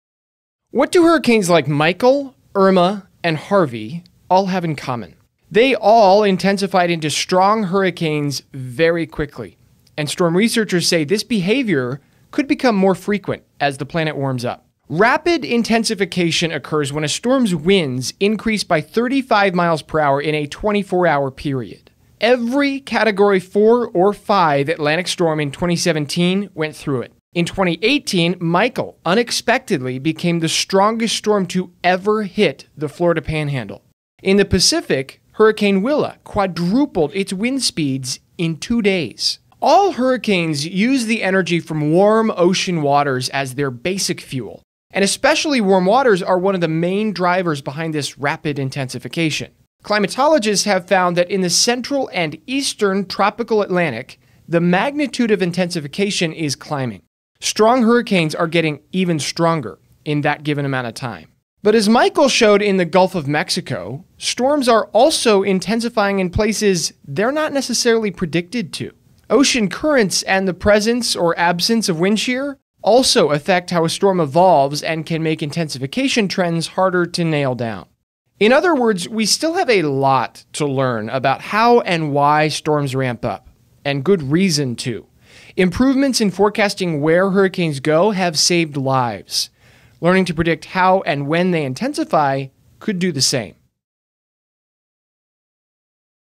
Rapid intensification is getting more severe
Hurricanes could get more severe more quickly than before, according to the scientists who study storm trends and ocean temperatures.